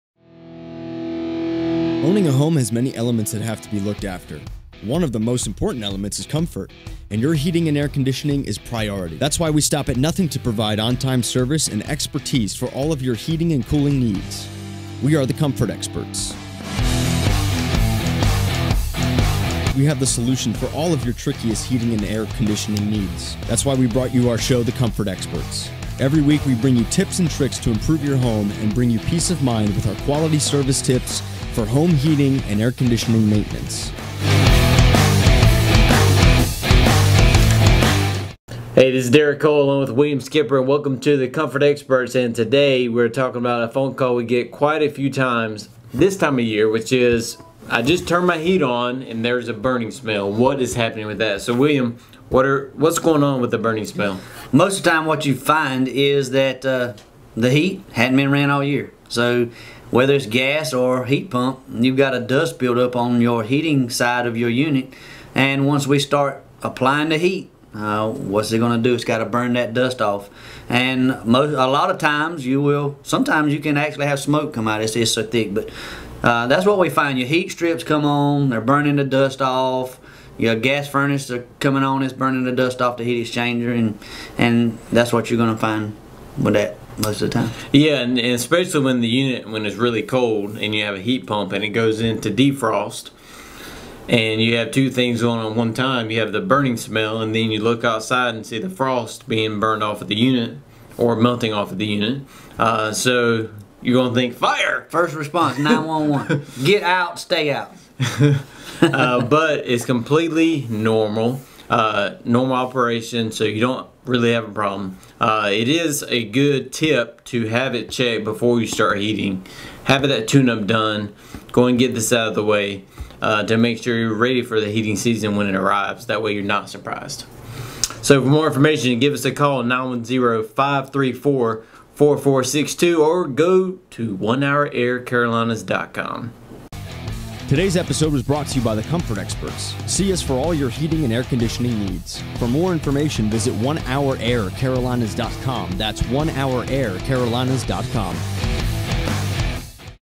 The Comfort Experts - Burning Smell
The Comfort Experts - Derek Cole & William Skipper cover what causes the burning smell when your heat first comes on.